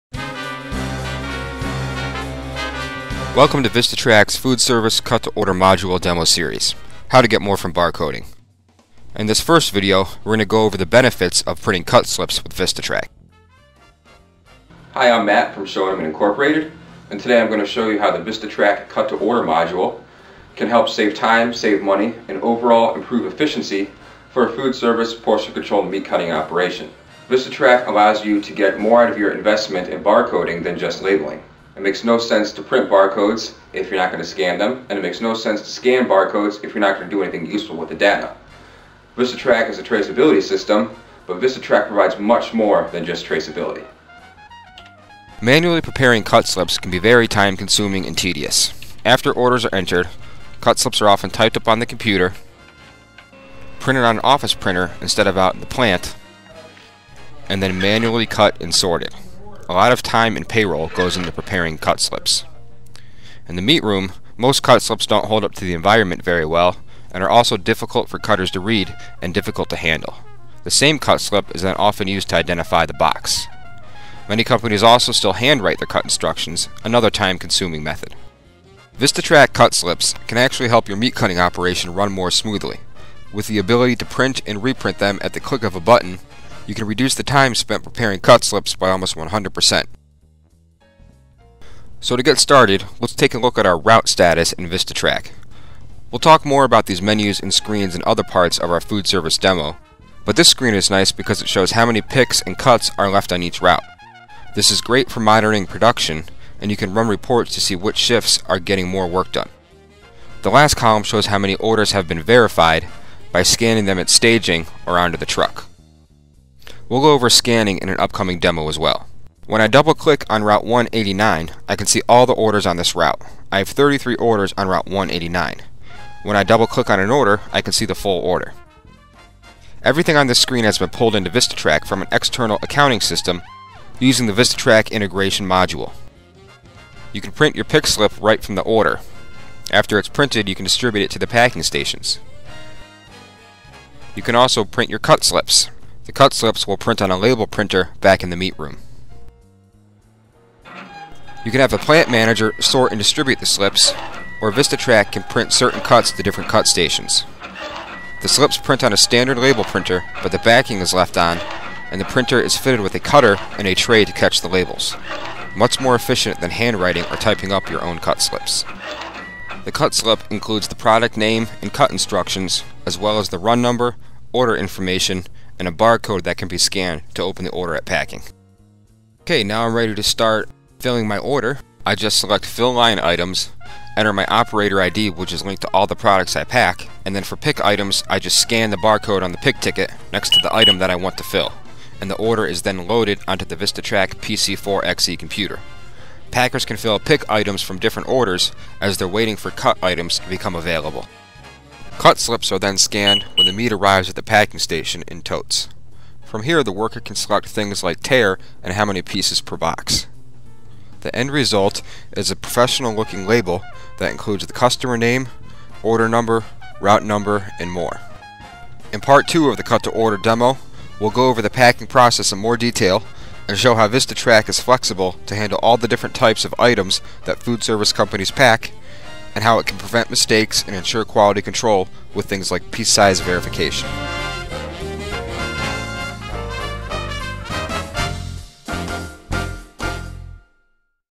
VistaTrac Food Service Cut to Order Module: Part 1, Cut Slips
This video demonstrates how to use the VistaTrac system for food service and cut-to-order operations. It shows how to print a cut slip from the system, which guides a worker through the cutting process based on specific orders.

The video focuses on the software's ability to help plant-floor workers manage orders efficiently and accurately. By using a digital system, businesses can eliminate manual, paper-based processes, reducing errors and improving overall efficiency. This is a crucial step for maintaining full product traceability and ensuring an accurate audit trail throughout the supply chain. The use of barcoding and a user-friendly interface simplifies complex tasks for the plant-floor worker.